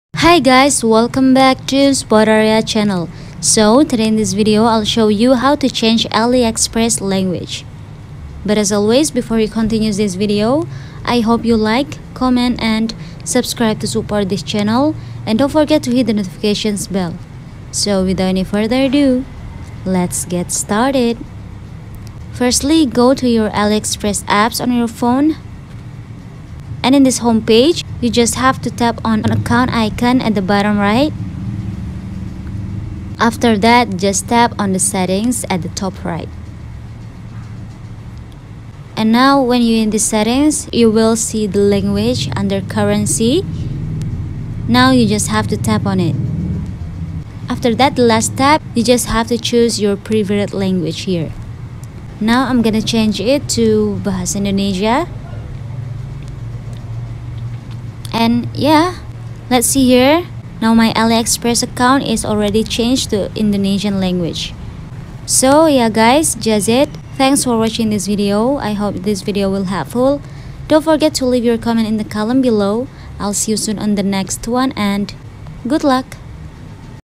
How To Change Aliexpress Language 2022
Hi guys, in this video I'll show you how to change aliexpress language 2022.